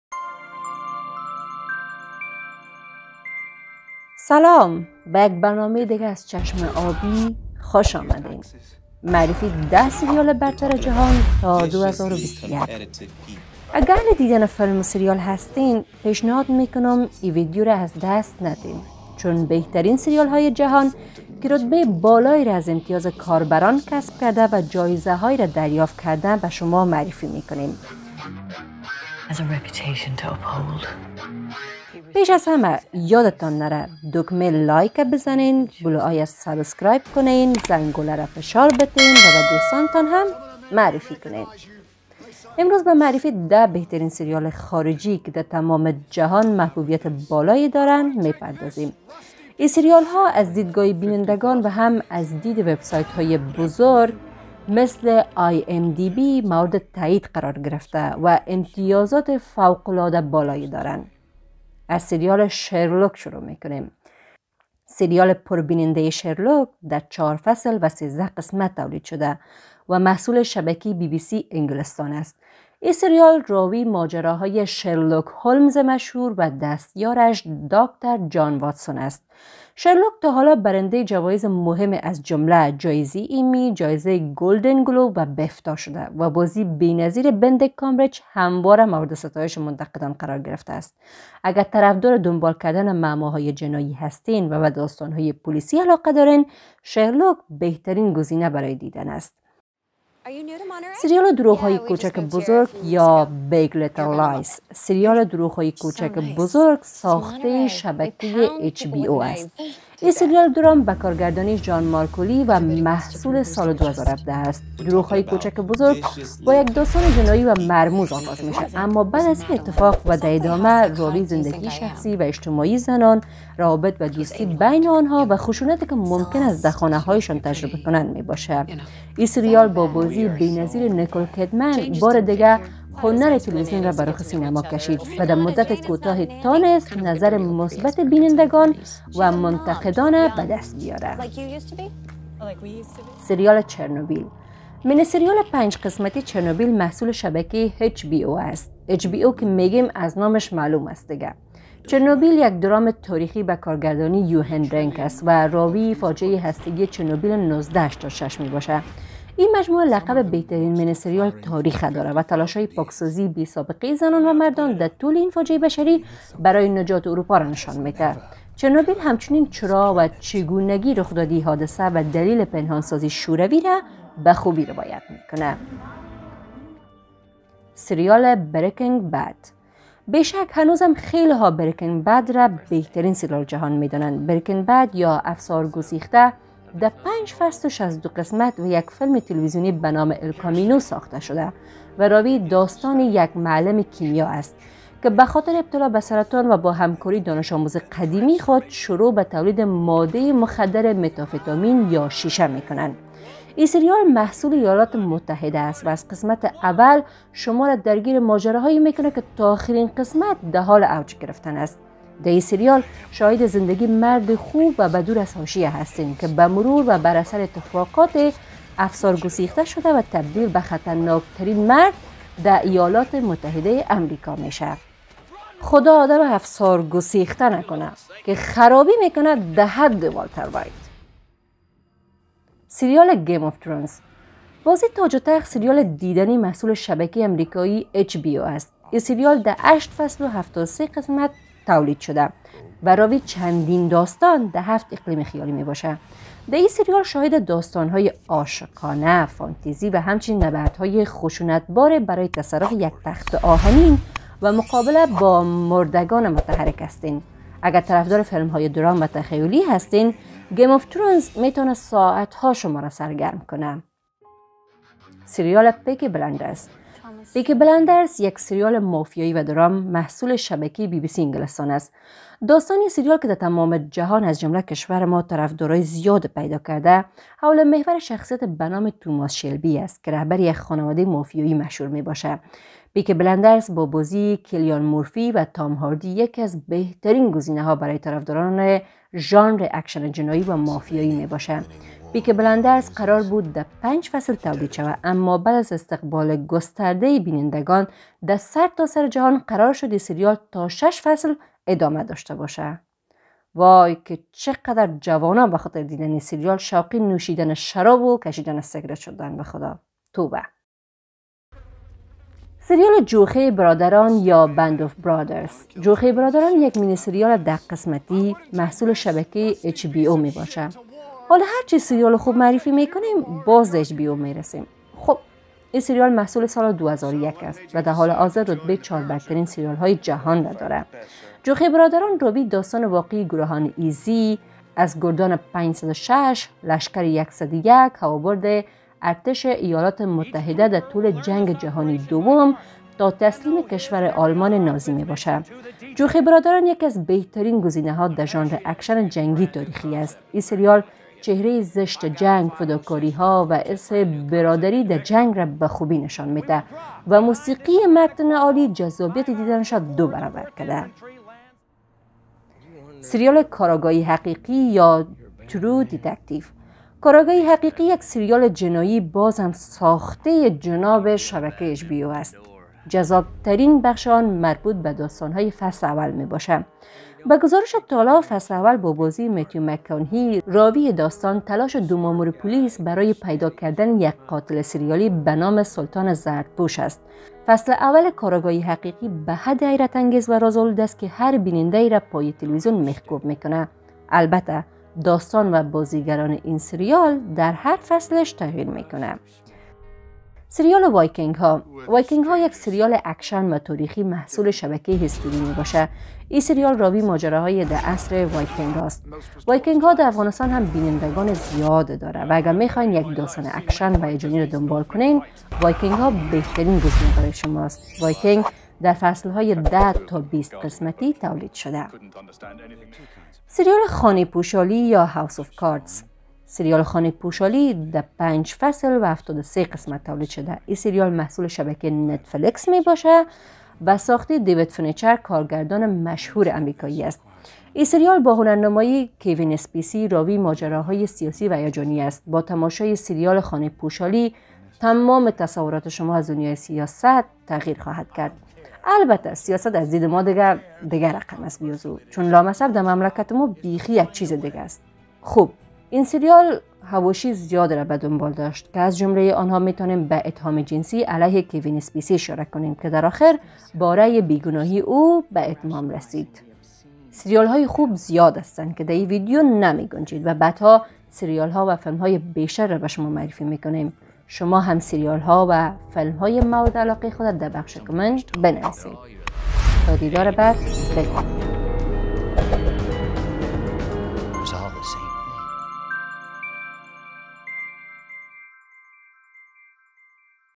ده سریال برتر جهان تا 2021
با ده سریال برتر جهان تا سال 2021 آشنا شوید

Re-uploads of this content will be blocked!